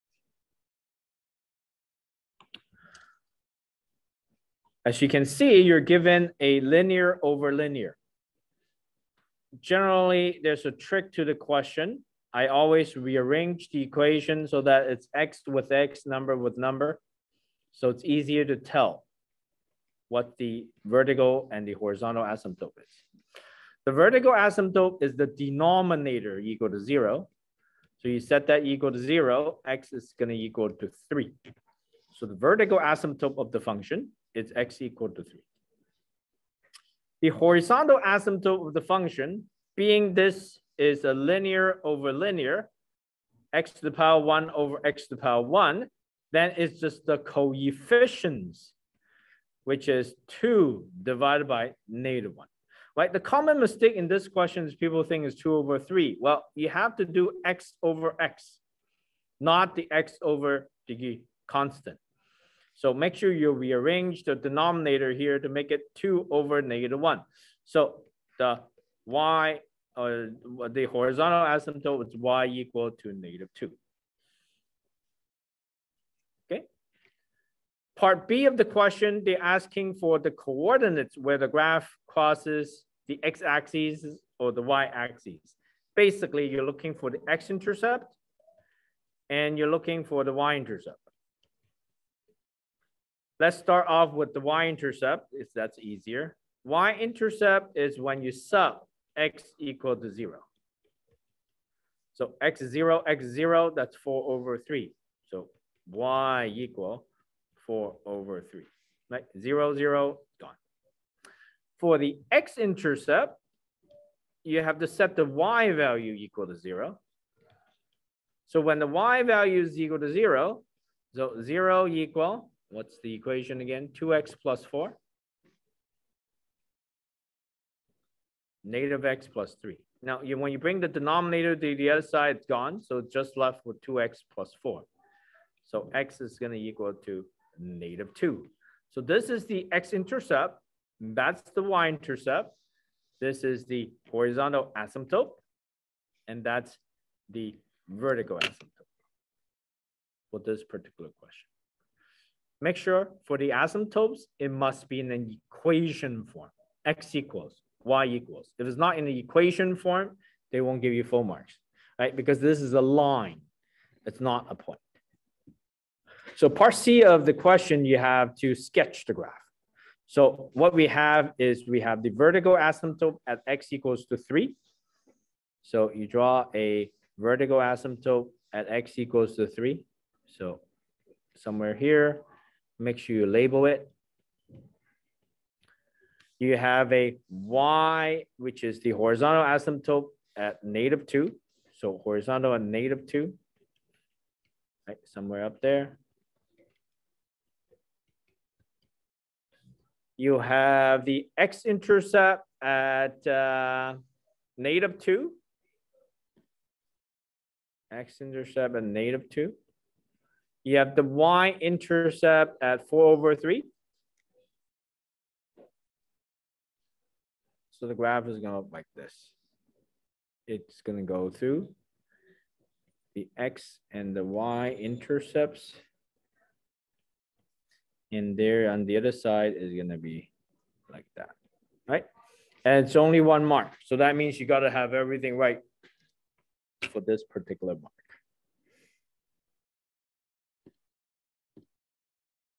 Nov21 IB Math AA SL Paper 1 #3
I'm going to take up the Nov 21 Math AA SL Paper 1.  Here's the solution to #3.